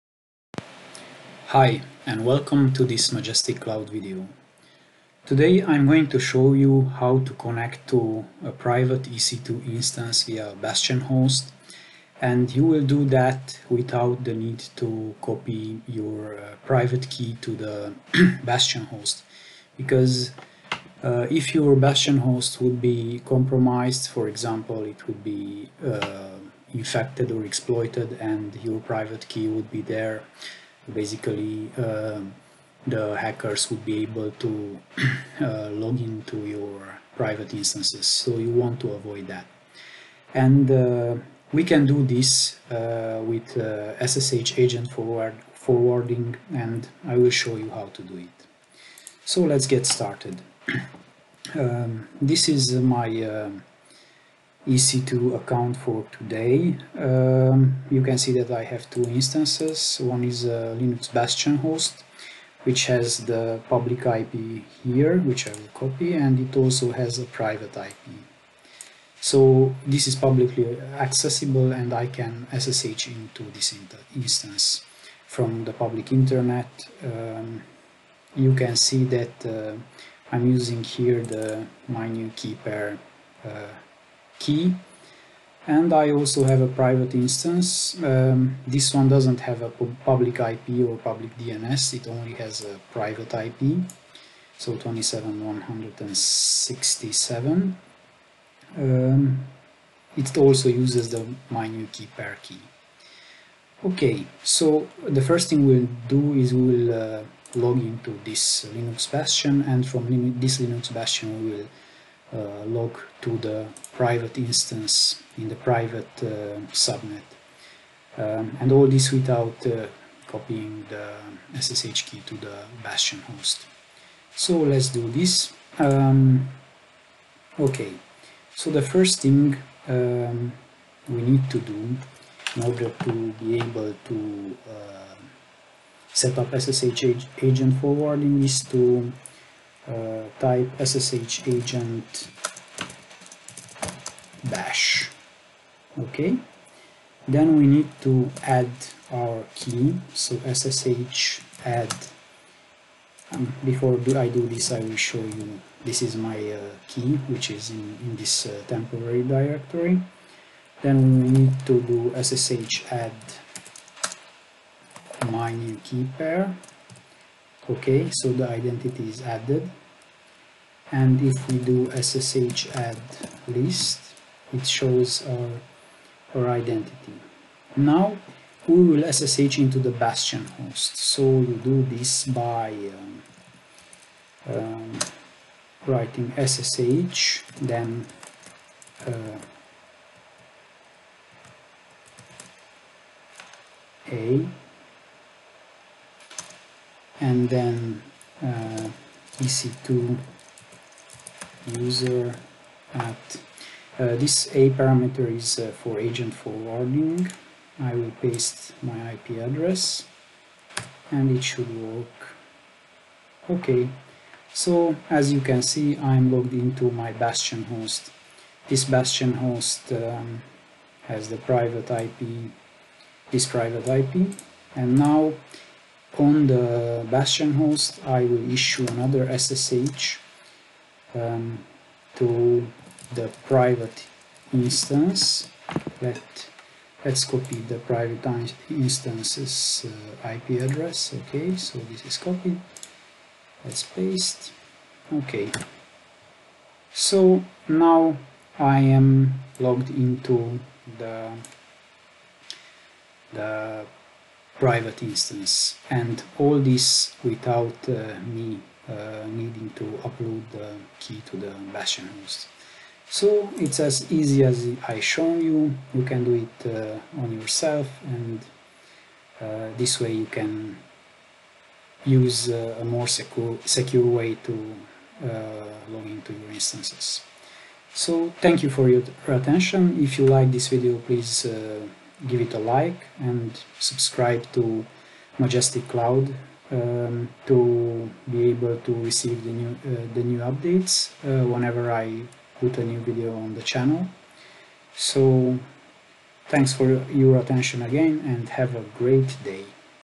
Connect to private instances via bastion hosts with SSH agent forwarding
To connect to a private EC2 instance you need to use a bastion host (oka jump box). But because of security concerns you might not want to upload your private key to the bastion host. How do you solve this dilemma ? Easy! With SSH Agent forwarding. This video shows you how to do that quick and easy.